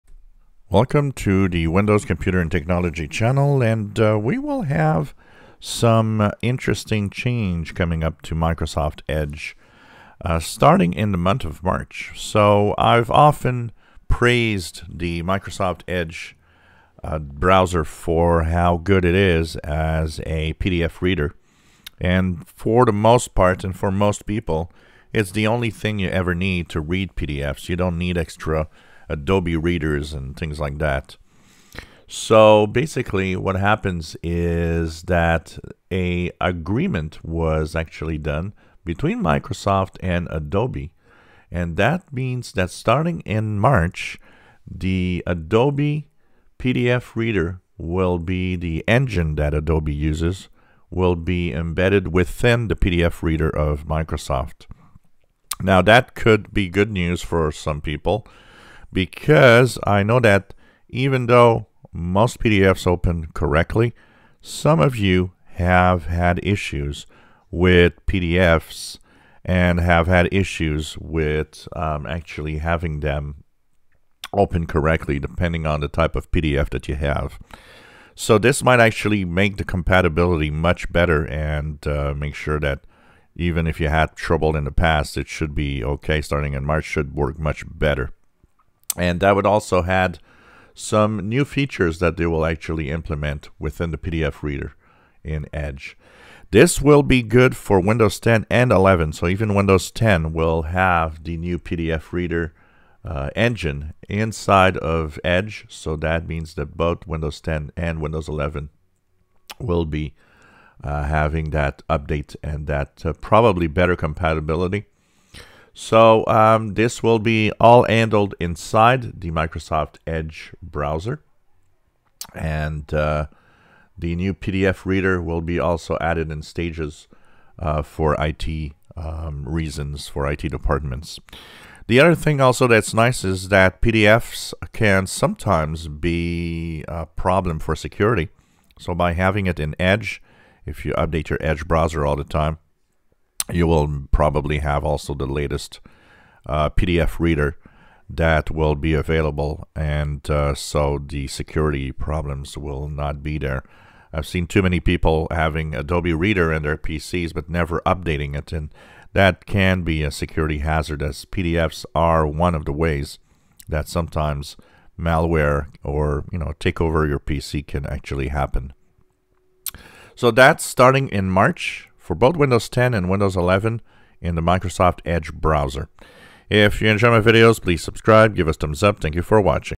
Microsoft EDGE Will get Adobe PDF Rendering engine in March 2023
Microsoft and Adobe have come to a agreement to implement the PDF engine within Edge on both Windows 10 and Windows 11